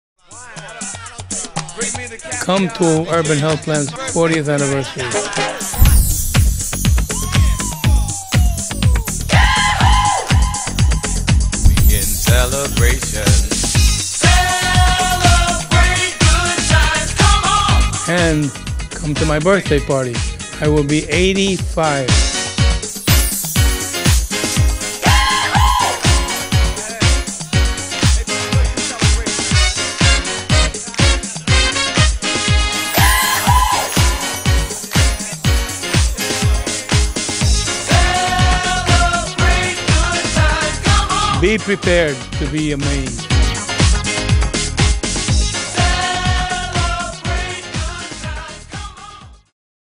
SAVE THE DATE - October 23, 2014
Join Urban Health Plan as we celebrate our 40th Anniversary on October 23, 2014 at Split Rock Golf Course.